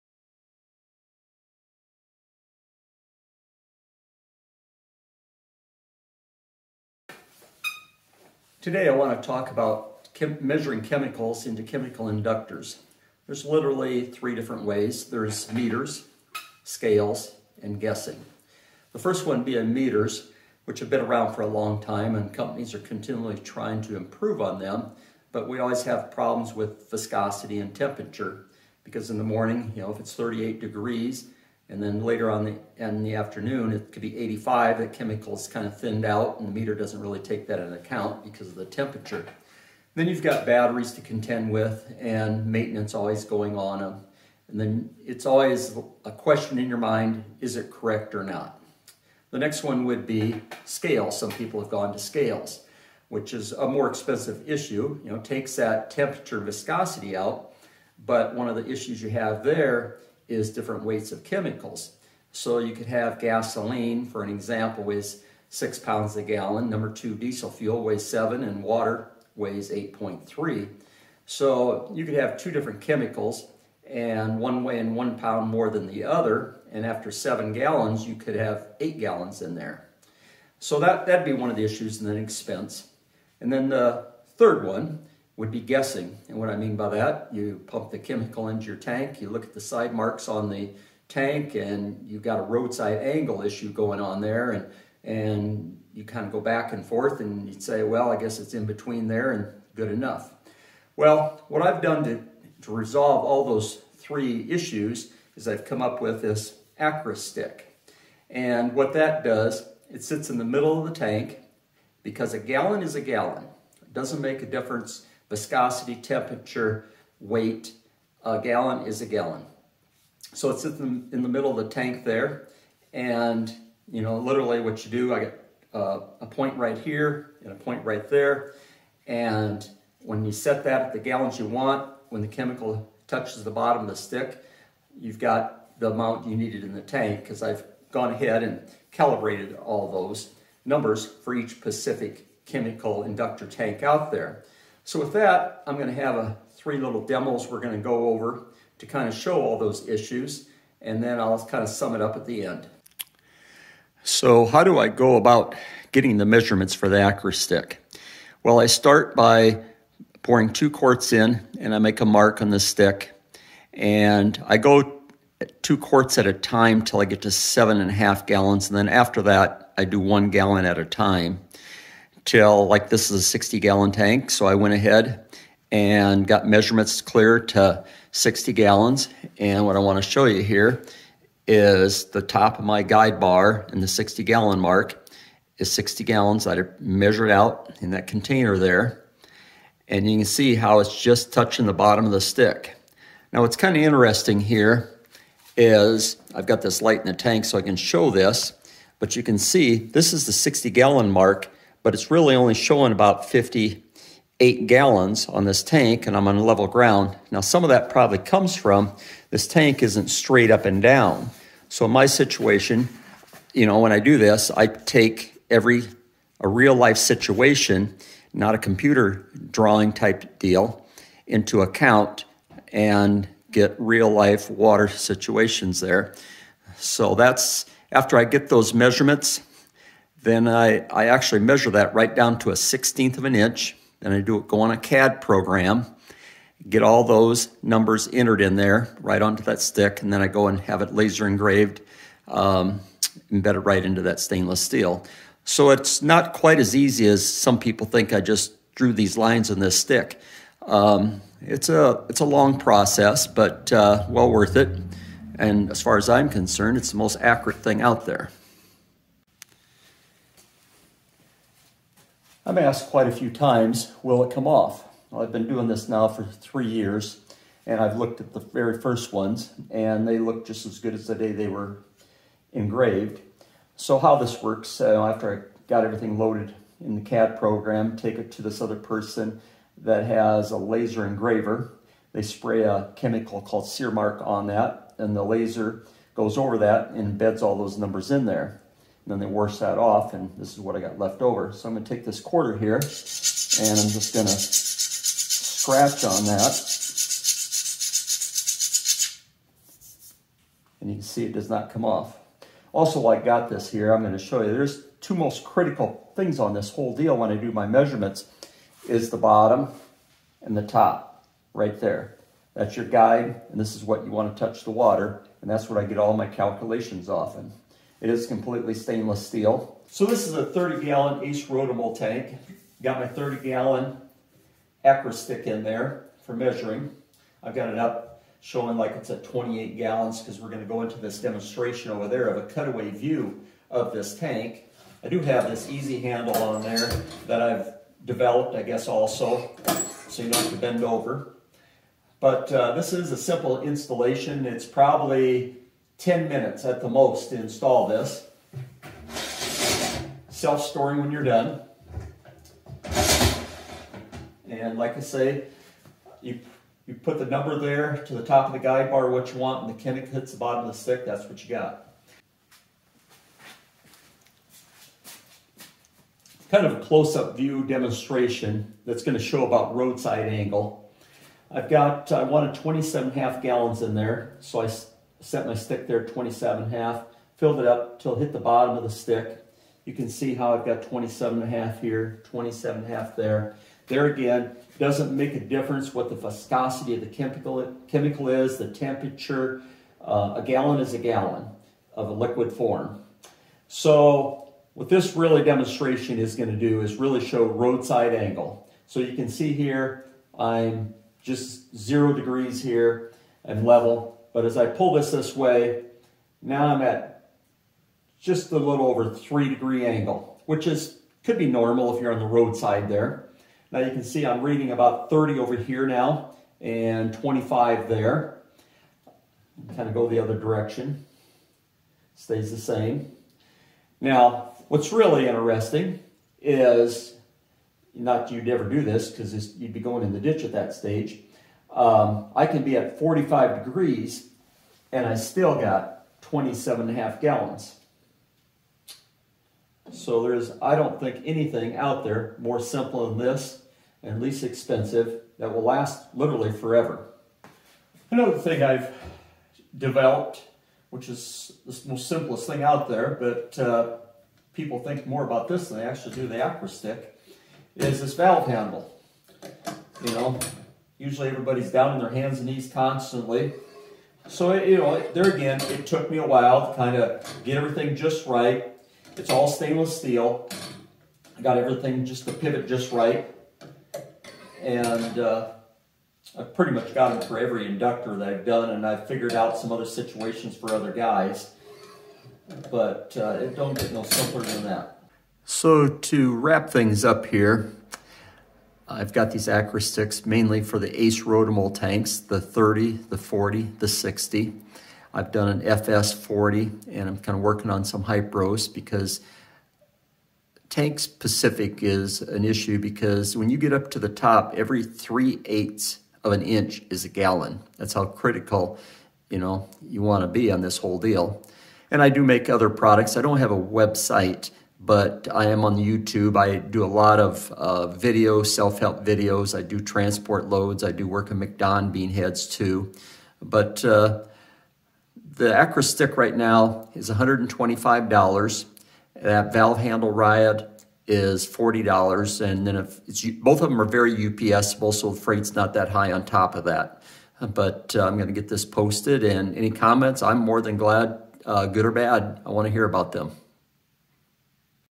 Randy Loder Chemical Inductor Measuring
The Accurate Stick is the simplest and lowest cost measuring device out there. Completely stainless steel, no batteries, no winterizing, not affected by tempature, viscosity,